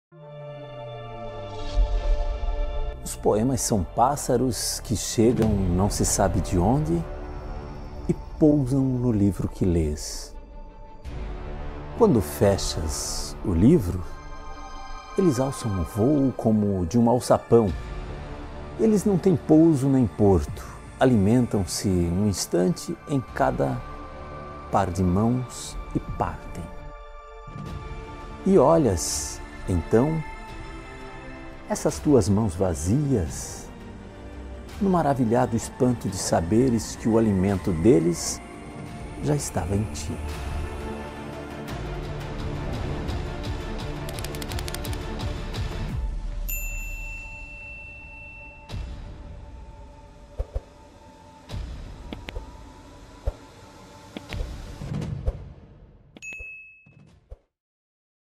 Martos | OS POEMAS | de Mário Quintana | Poesia falada.
Os poemas são como pássaros!?
É essa comparação que o poeta Mário Quintana faz neste lindo poema.
Se gostou compartilhe com alguém, curta e deixe seu comentário.